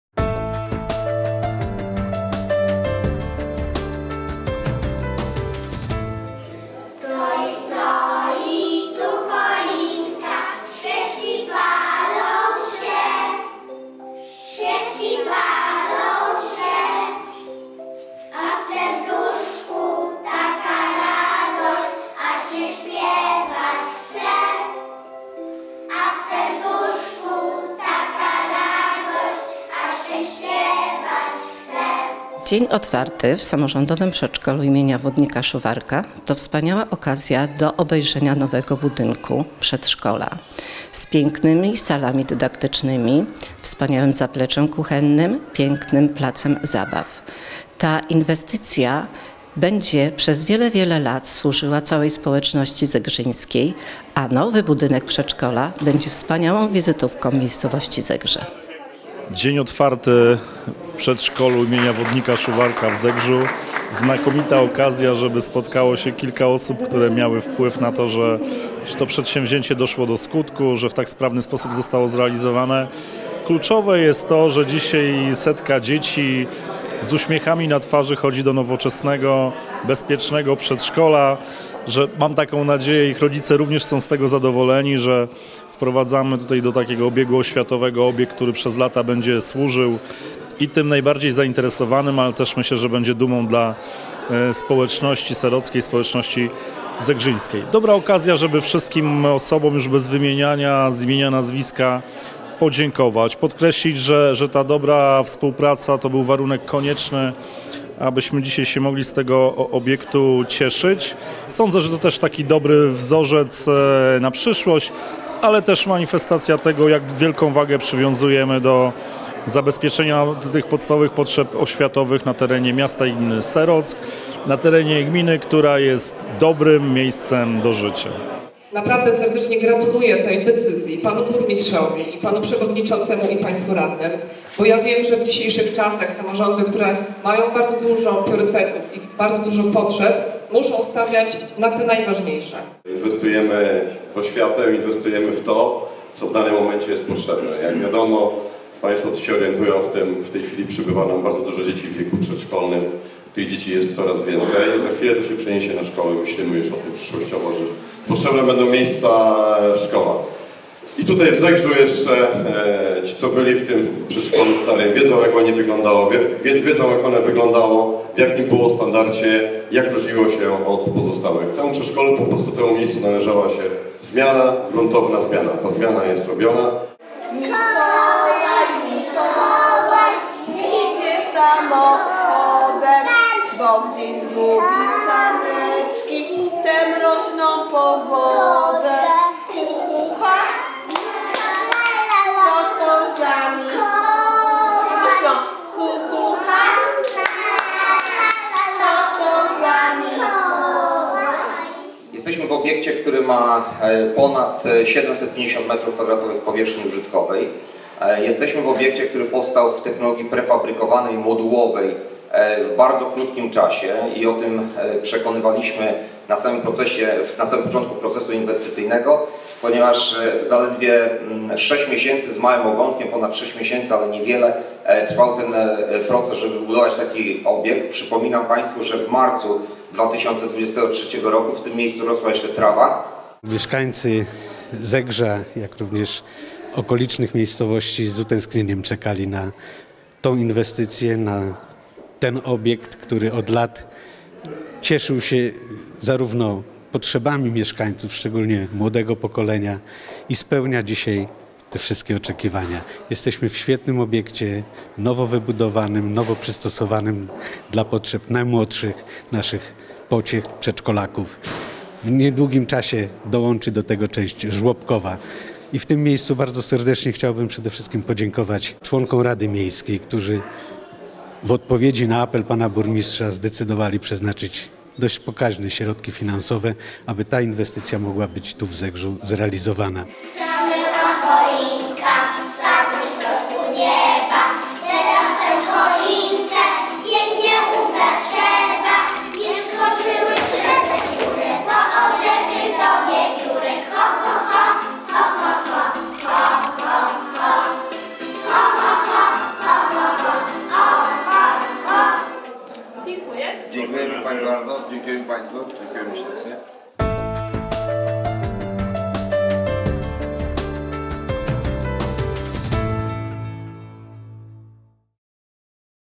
Wodnik Szuwarek w nowym budynku
7 grudnia w samorządowym przedszkolu w Zegrzu odbył się dzień otwarty. Uroczystość stała się okazją do zaprezentowania zgromadzonym nowego budynku i opowiedzenia o planach otwarcia obok pierwszego gminnego żłobka.